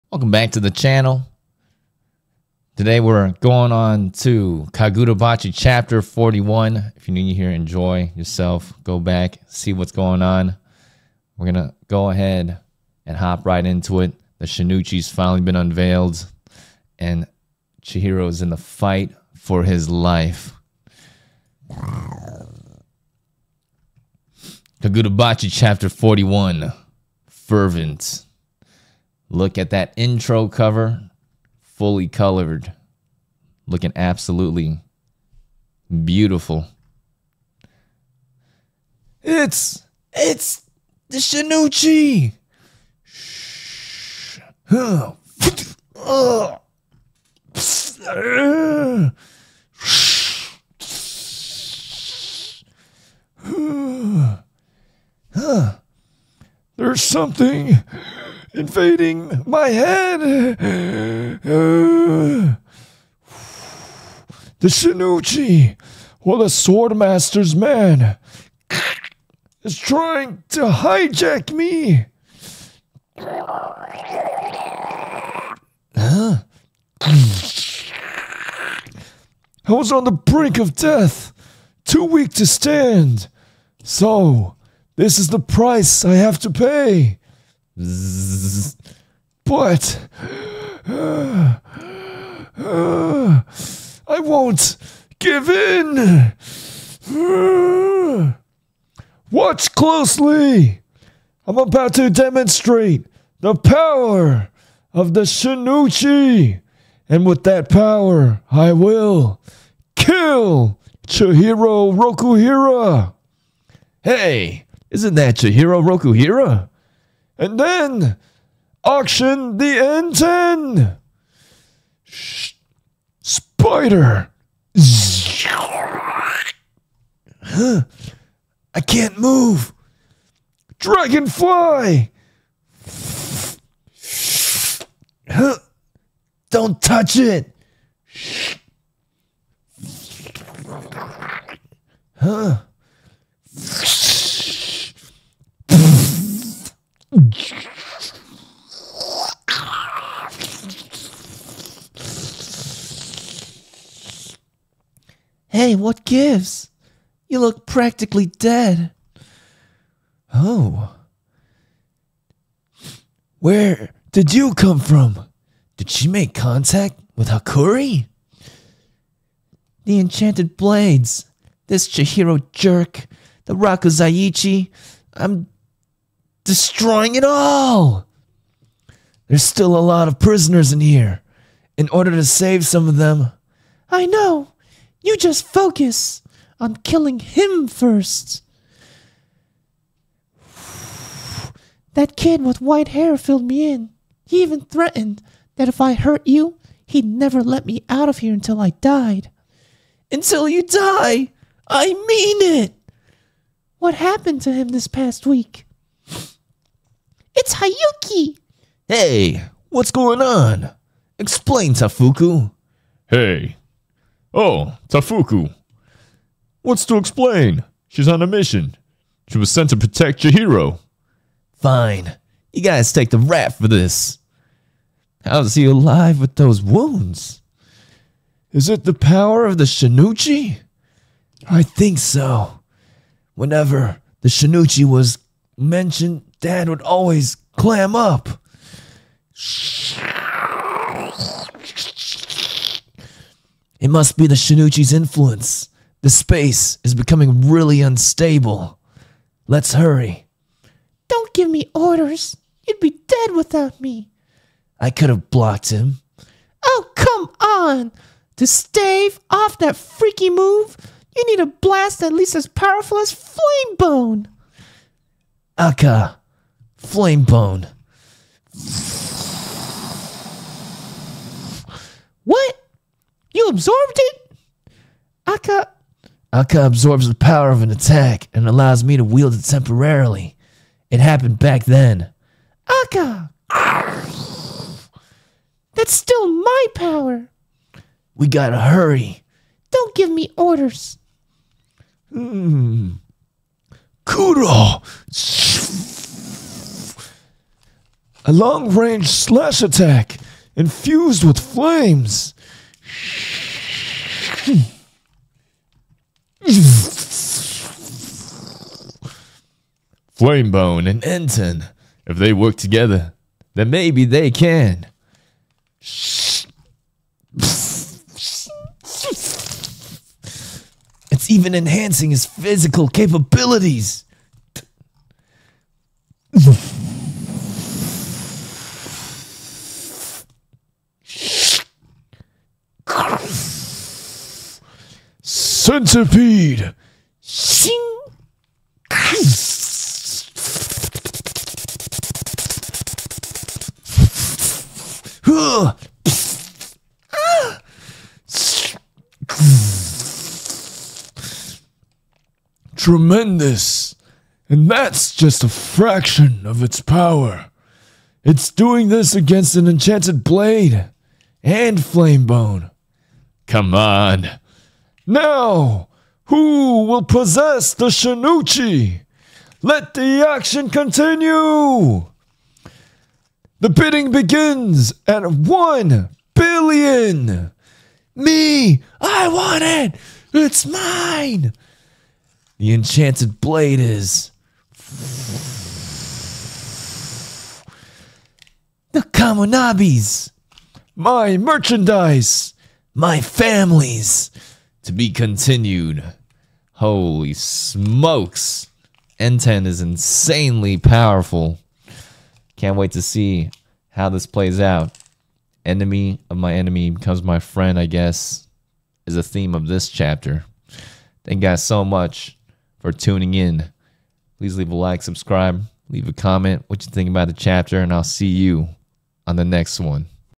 Fervent
Kagurabachi Chapter 41 Fervent

Kagurabachi is about Chihiro Rokuhira, the son of a renowned blacksmith who forged six enchanted swords, seeks a bloody revenge against a gang of sorcerers with the help of a seventh enchanted sword forged by his father before he was assassinated.

⦊ EPIC CODE: KIKIKATME

Live Monday - Friday 5PM PST